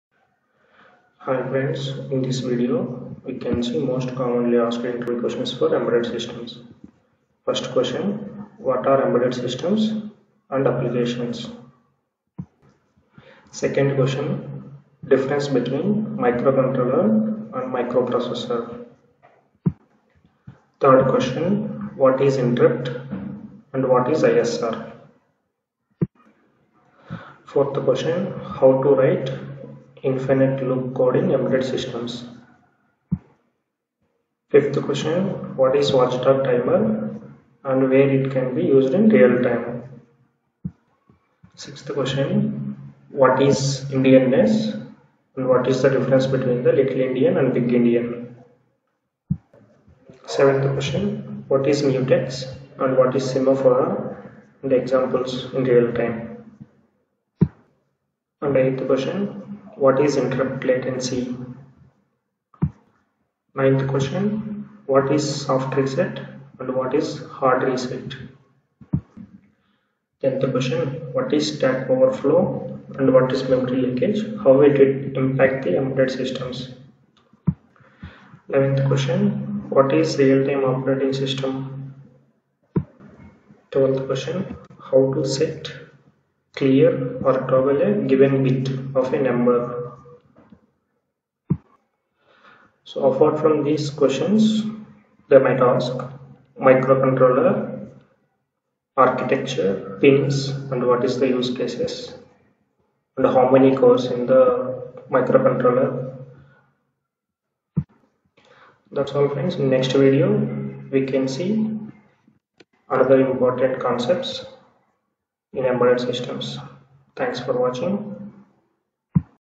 Embedded Systems Interview Questions for freshers and Experience
Most Commonly asked Embedded Systems Interview Questions.
This Channel also provide content related to  AUTOSAR, CAN, EMBEDDED SYSTEMS, IMPORTANT INTERVIEW Questions.